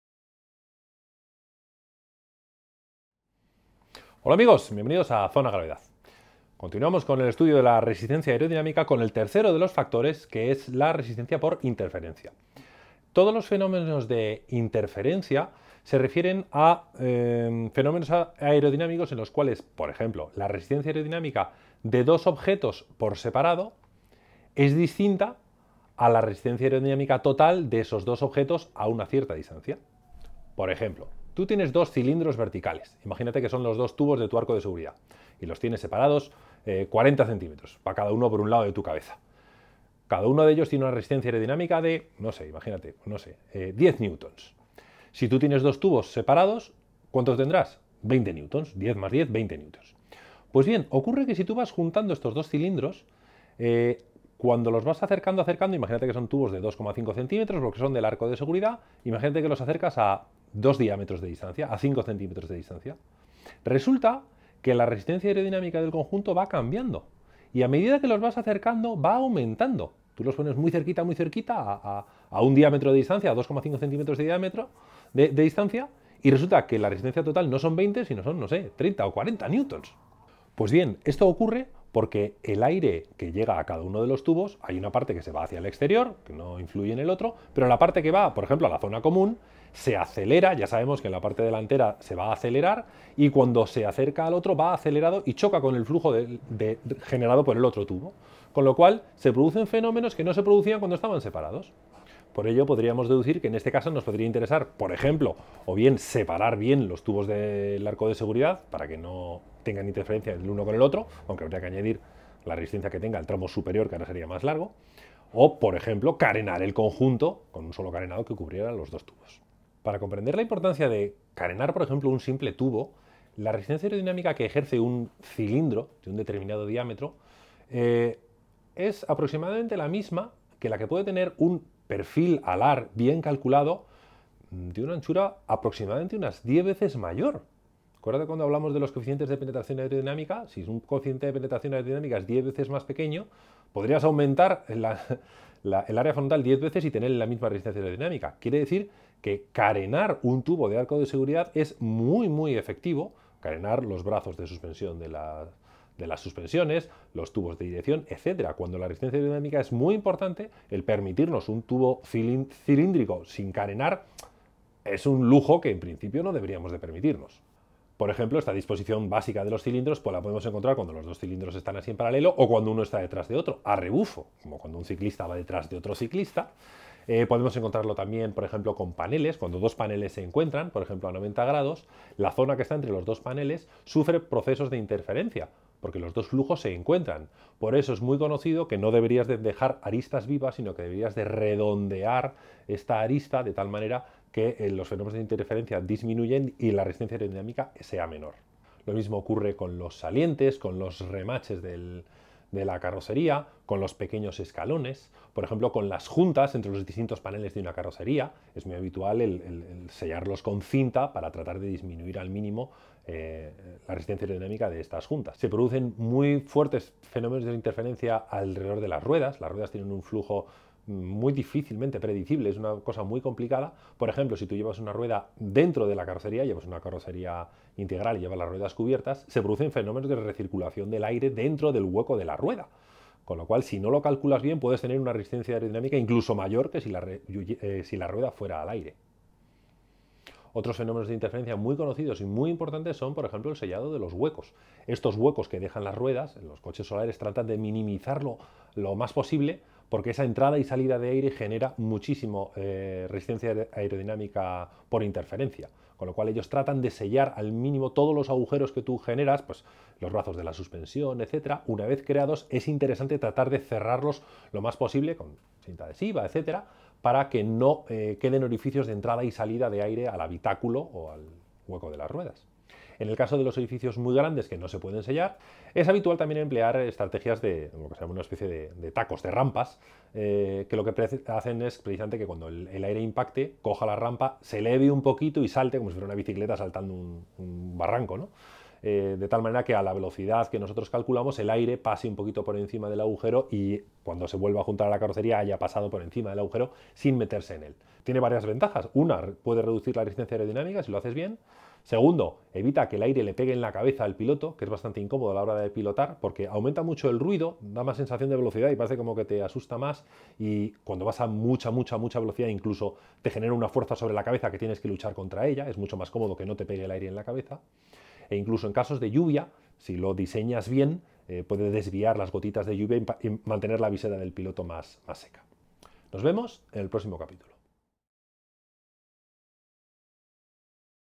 Dinámica 168: Resistencia por interferencia
La resistencia aerodinámica de dos cuerpos depende de su posición y la distancia que los separa.
El sellado de juntas y aberturas, el redondeado de las esquinas o el empleo de un derivabrisas puede disminuir notablemente la resistencia aerodinámcia.